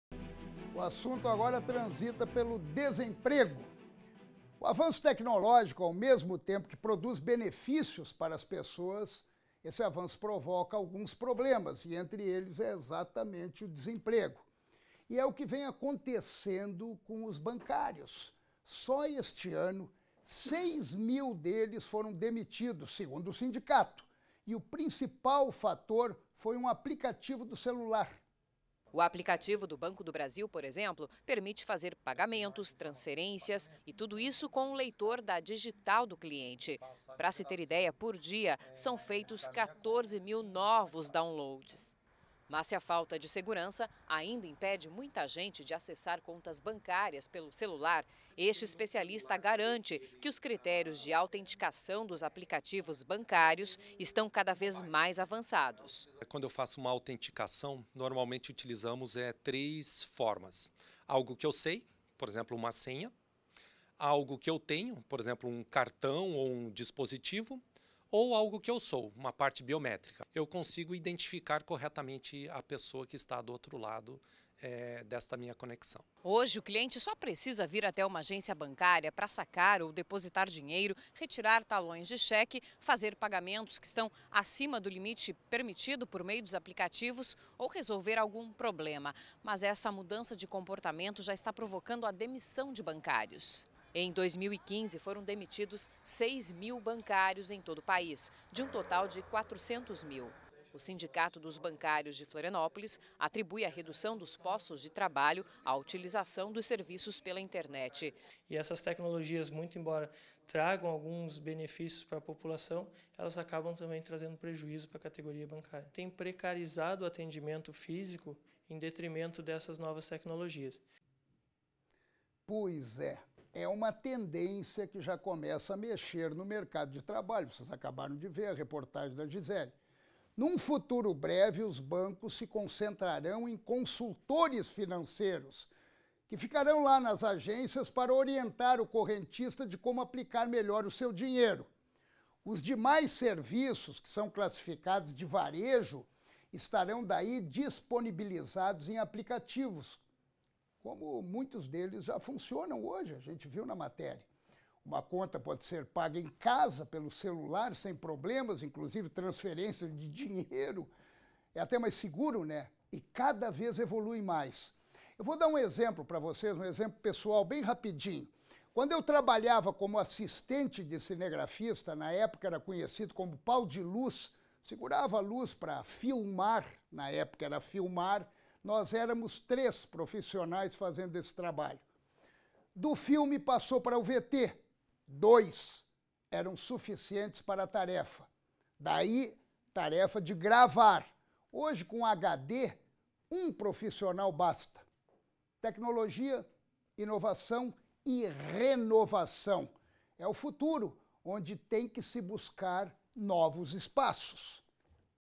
Aplicativos facilitam uso de serviços mas provocam demissões de bancários
O avanço técnologico ao mesmo tempo que produz benefícios para as pessoas, provoca alguns problemas, entre eles, o desemprego, situação que vem acontecendo com os bancários. Só este ano 6 mil deles foram demitidos, segundo o sindicato.  O principal fator foi um aplicativo do celular.